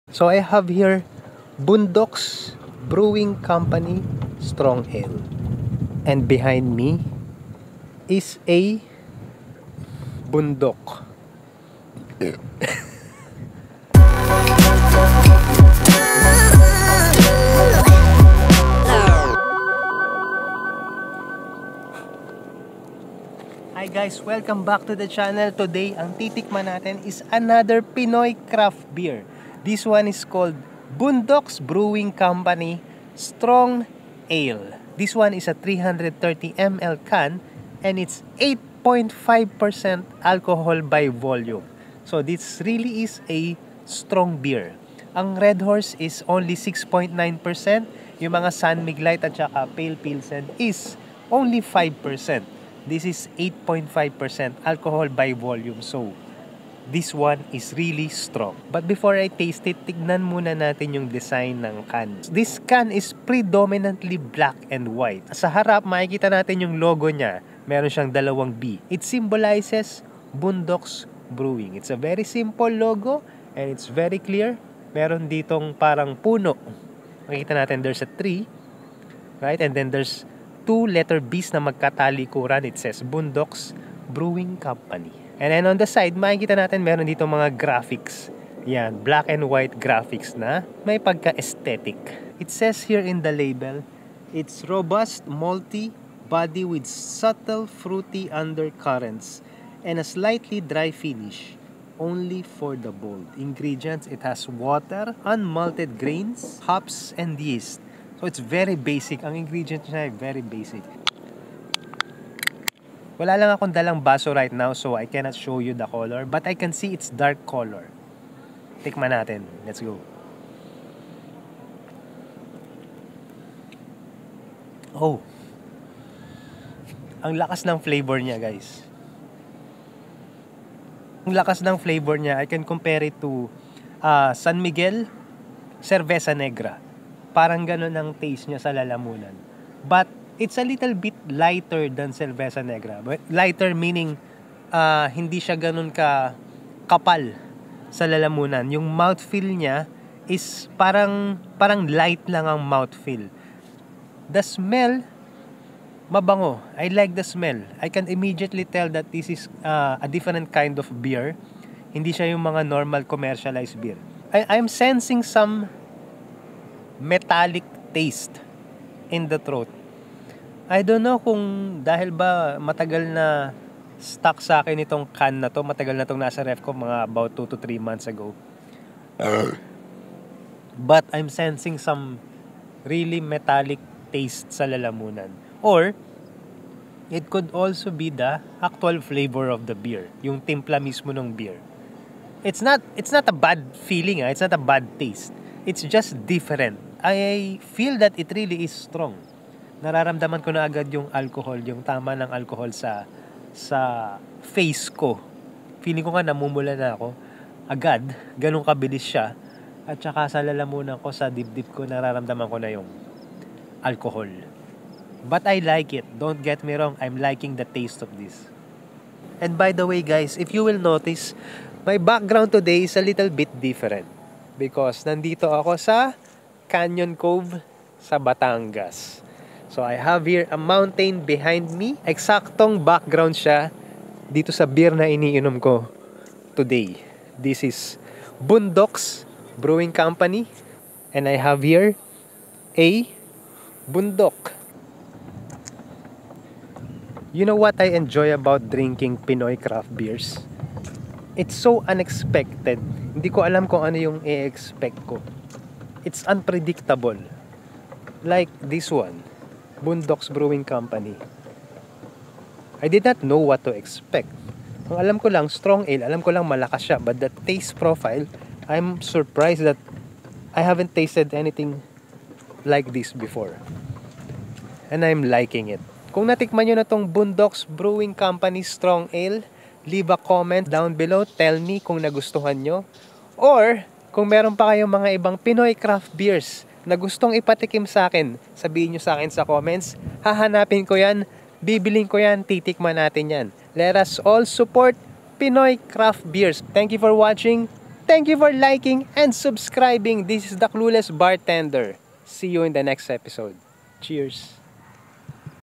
Lets try this STRONG ALE - Pinoy Craft Beer by Boondocks Brewing Co
Heres another Pinoy Craft Beer - this 8.5 percent abv beer really is strong. Check out Boondocks Brewing Co on facebook
Todays location : Canyon Cove Batangas